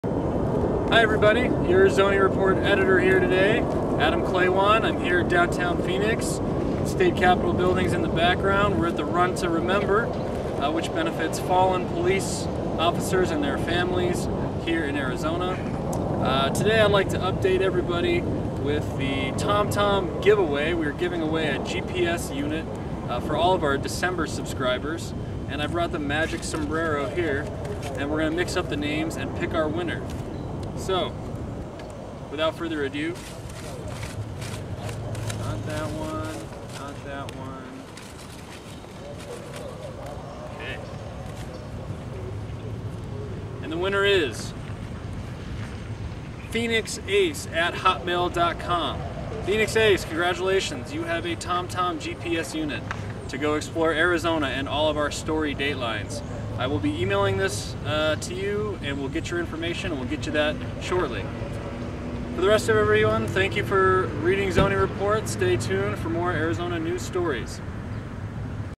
GPS giveaway!
TZR Editor Adam Klawonn gives away a TomTom 125 GPS unit to lucky subscriber "phxace" of Phoenix in January 2009. Announcement is made at the Run to Remember in Downtown Phoenix, which benefits the families of fallen police officers. Klawonn ran a 5K in the event.